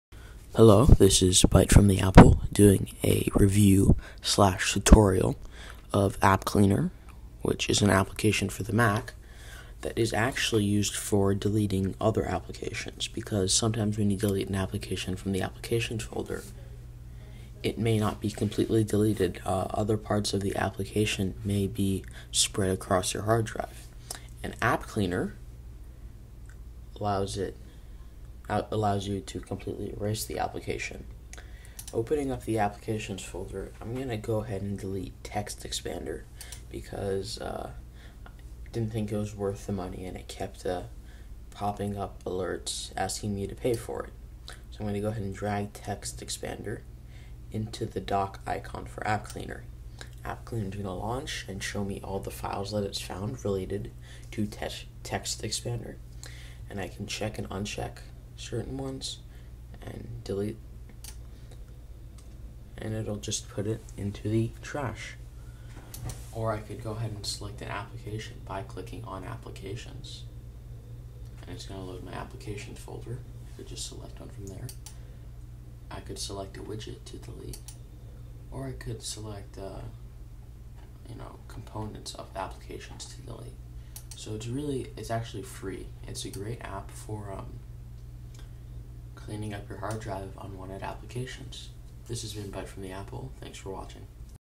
AppCleaner Tutorial/Review
App Cleaner allows you to clean unwanted apps off your Mac. Please consider donating to the developer for this fantastic, free app.